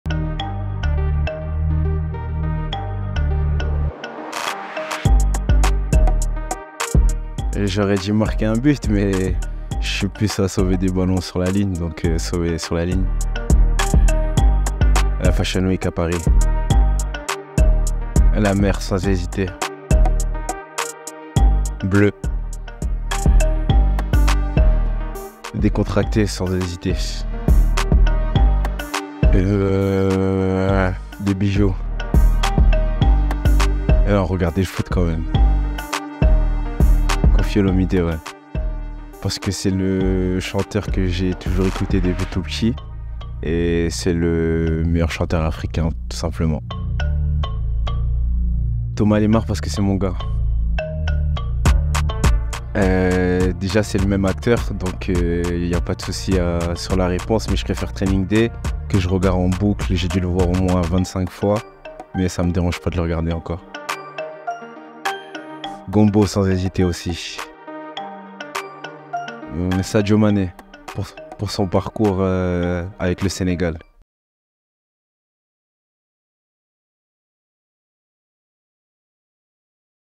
"Tu préfères" avec Kevin N’Doram
Grenat depuis 2019, Kevin N’Doram se dévoile dans la traditionnelle interview "Tu préfères » !